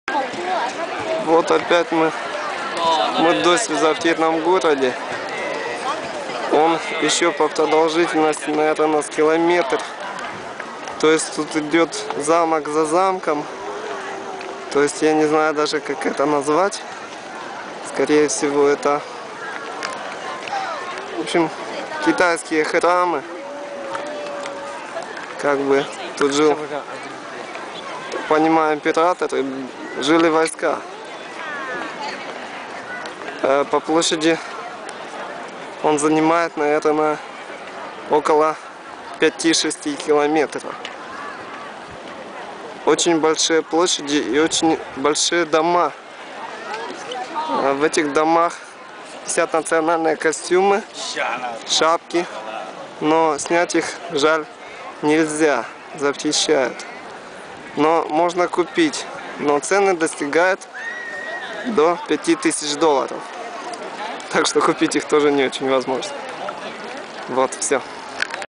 Gugong (Forbiden city) in Beijing, China
The Forbidden City was the Chinese imperial palace from the Ming Dynasty to the end of the Qing Dynasty. It is located in the middle of Beijing, China, and now houses the Palace Museum. For almost five hundred years, it served as the home of emperors and their households, as well as the ceremonial and political center of Chinese government.
Built in 1406 to 1420, the complex consists of 980 buildings with 8,707 bays of rooms[1] and covers 720,000 m2 (7,800,000 sq ft). The palace complex exemplifies traditional Chinese palatial architecture,[2] and has influenced cultural and architectural developments in East Asia and elsewhere. The Forbidden City was declared a World Heritage Site in 1987,[2] and is listed by UNESCO as the largest collection of preserved ancient wooden structures in the world.
Since 1925, the Forbidden City has been under the charge of the Palace Museum, whose extensive collection of artwork and artifacts were built upon the imperial collections of the Ming and Qing dynasties. Part of the museum's former collection is now located in the National Palace Museum in Taipei. Both museums descend from the same institution, but were split after the Chinese Civil War.